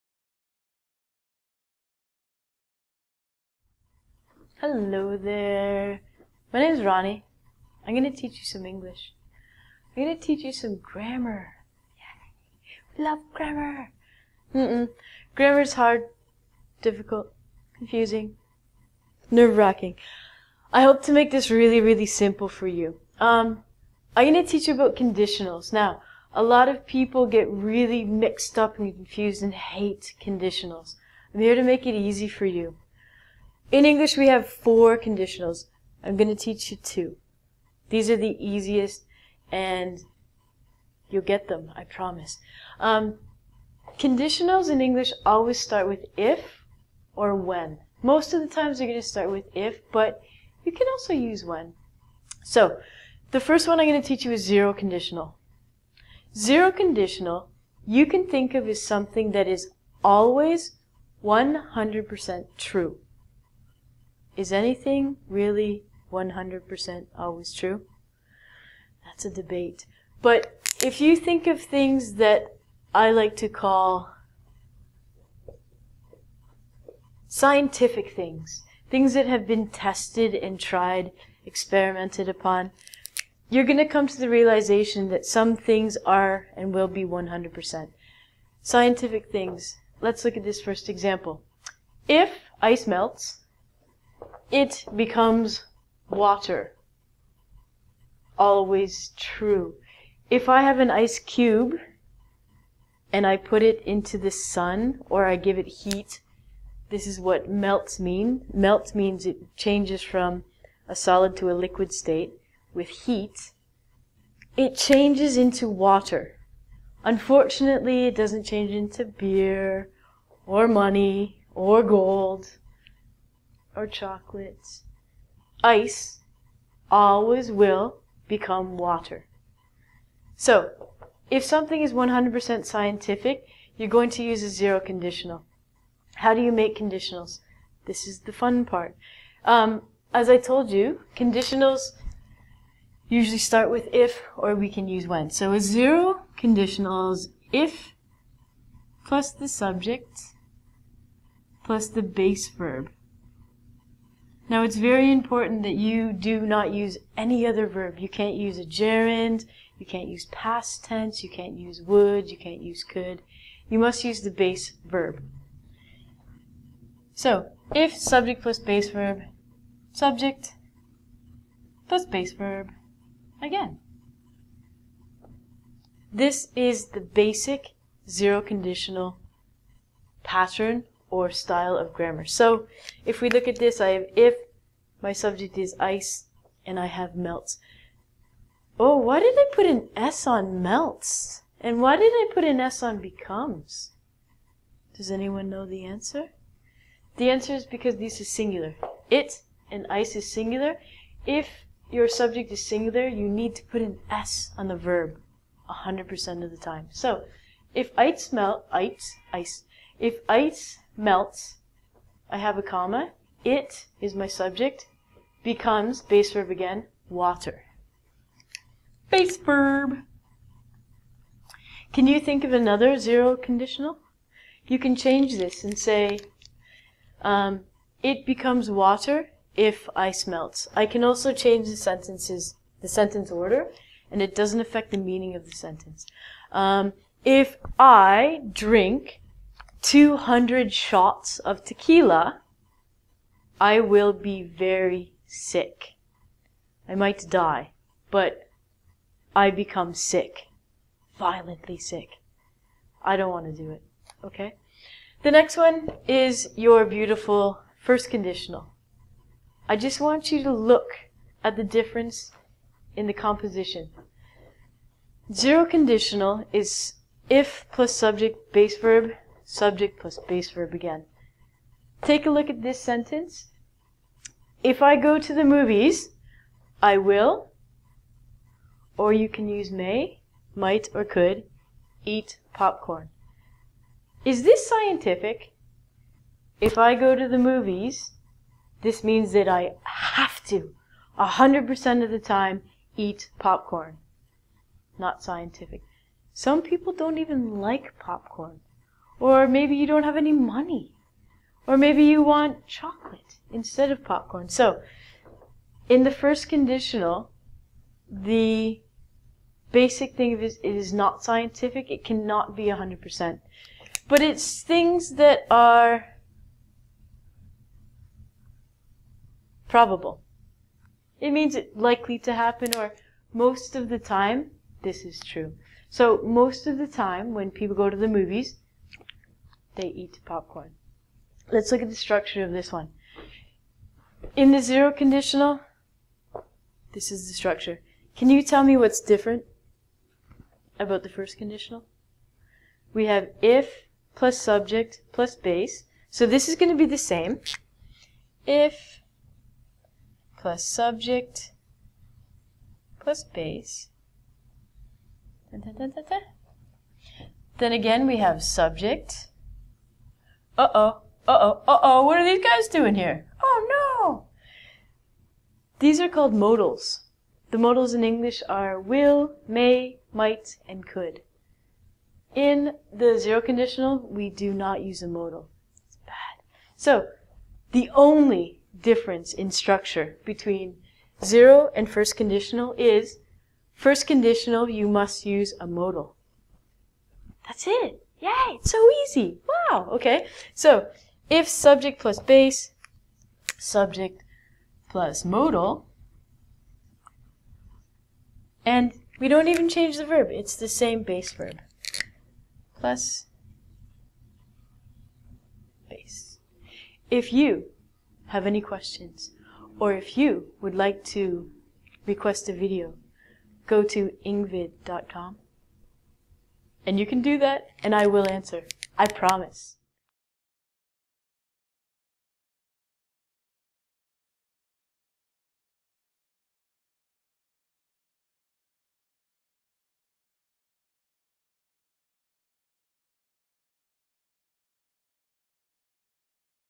Conditionals: zero & first conditionals (English Grammar)
"If I eat two hamburgers, I will be full." Conditionals in English grammar are very confusing! Learn what 'First Conditionals' and 'Zero Conditionals' are, when we use them, and how we use them correctly! I'll also teach you the differences between them, so you'll never confuse them again. You can take a free quiz on this lesson